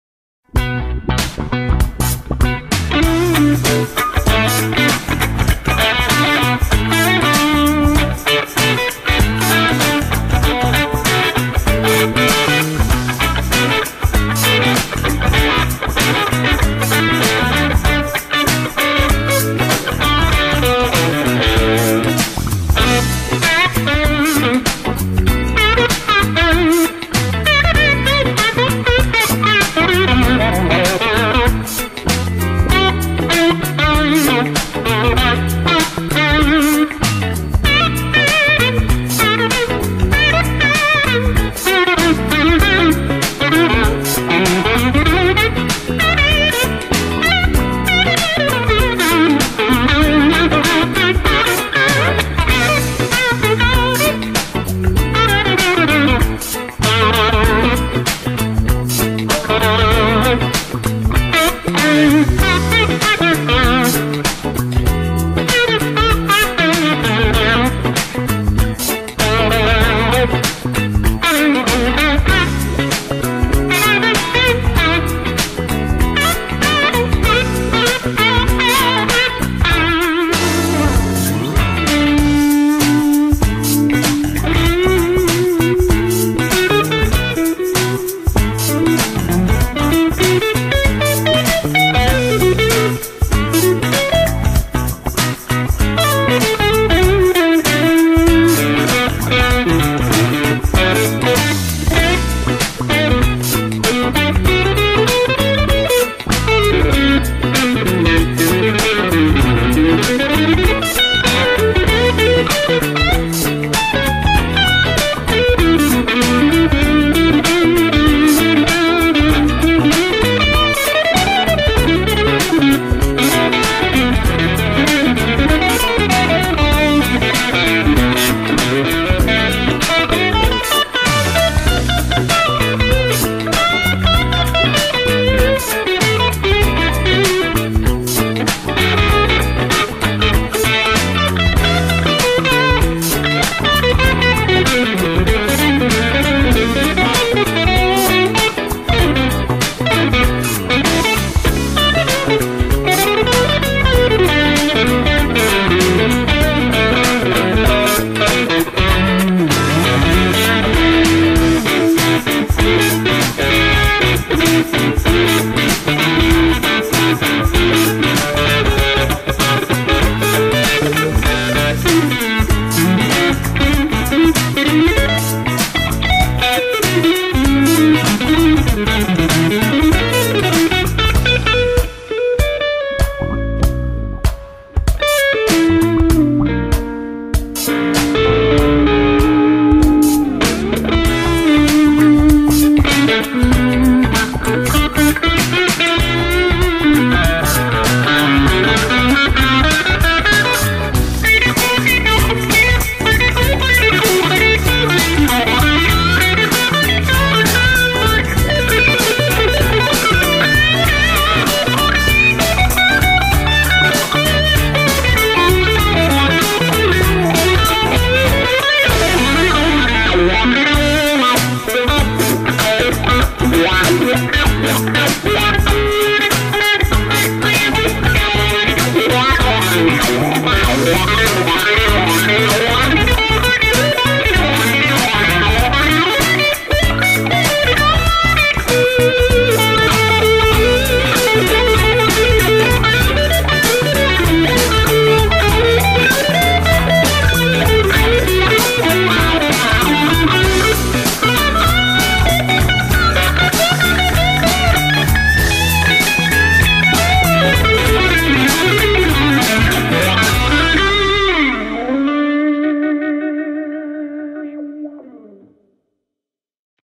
Fender Strat 97'/ Joyo JF-03 Crunch Distortion Drive Test
Fender American Standard Stratocaster 1997
Amp - Elmuz MV-3  (made in Poland) & cabinet with speaker JENSEN JC12 - 50 FA